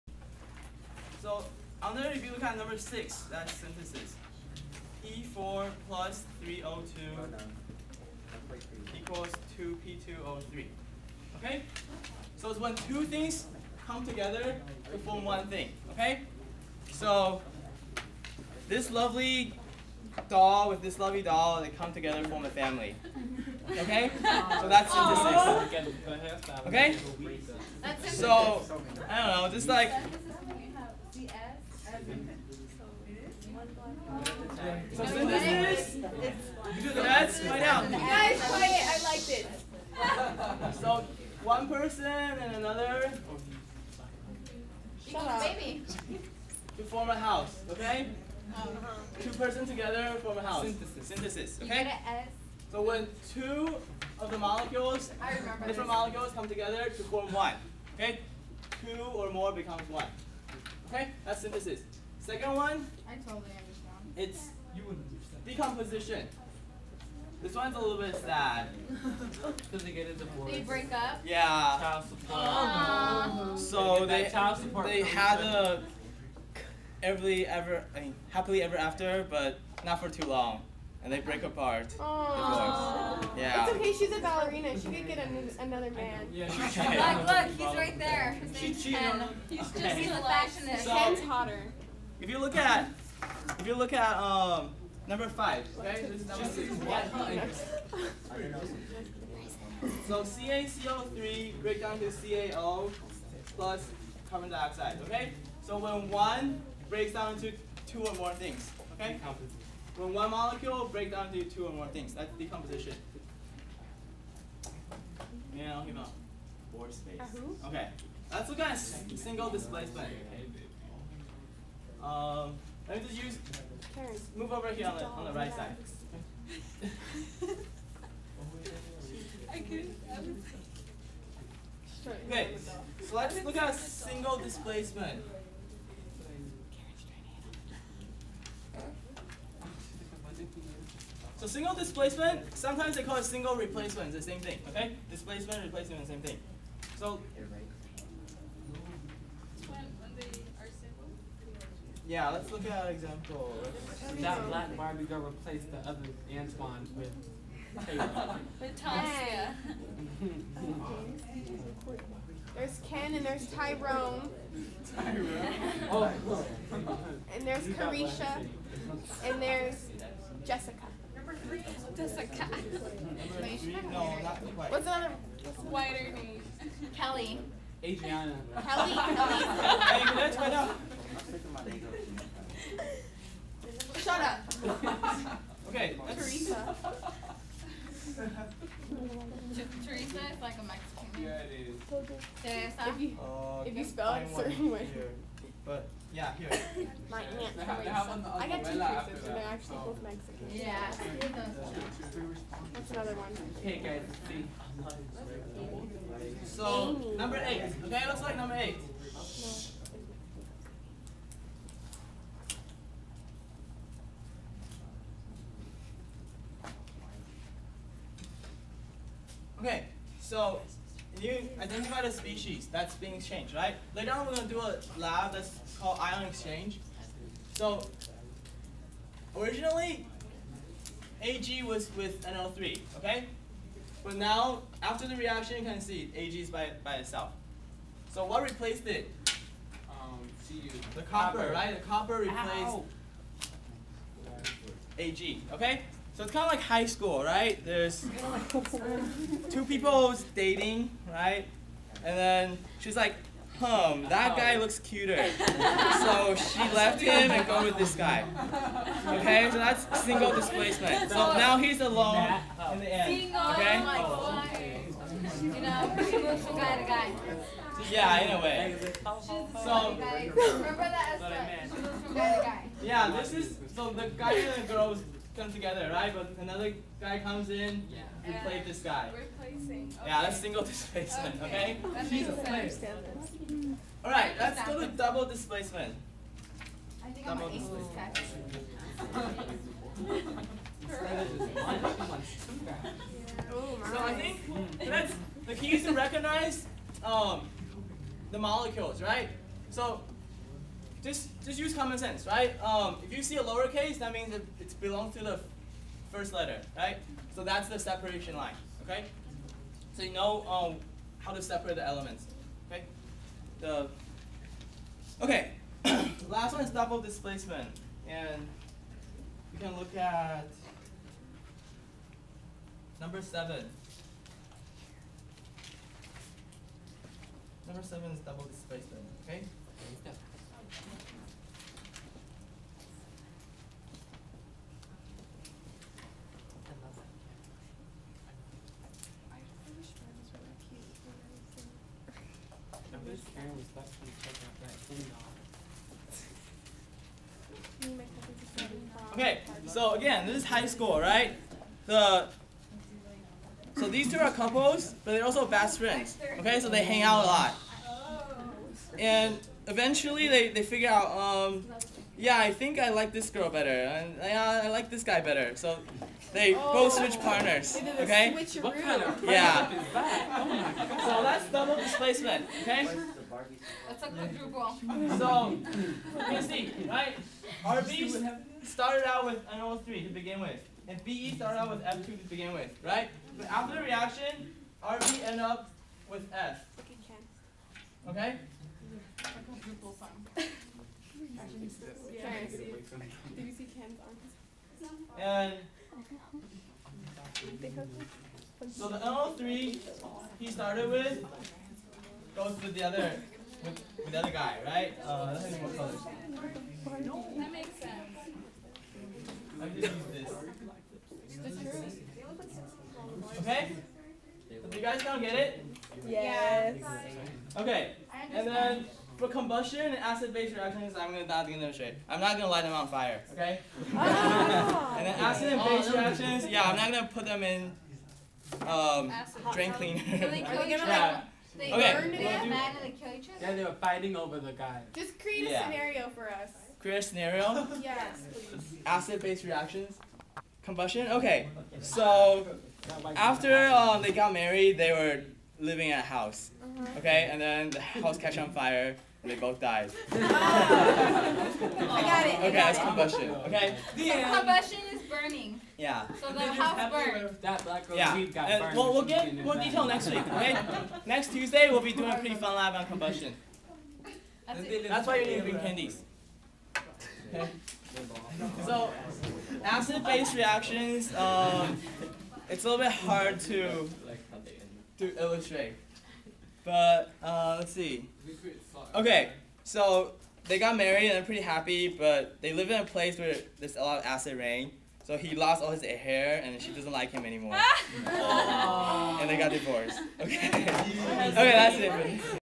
Types of reactions with Barbies.AVI
Teaching types of reactions (Synthesis, Decomp, Single Displacement, Double Displacement, combustion, and acid/base reactions.